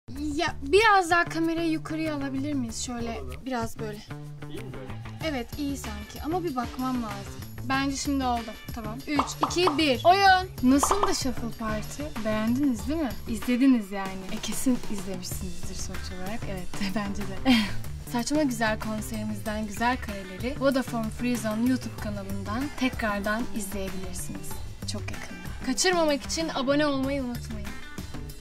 Shuffle Party kaçtı mı? Devamı var! | FreeZone Stüdyo
FreeZone Stüdyo'da Aleyna Tilki ile Shuffle Party online konserinin en iyi performansları çok yakında kanalımızda.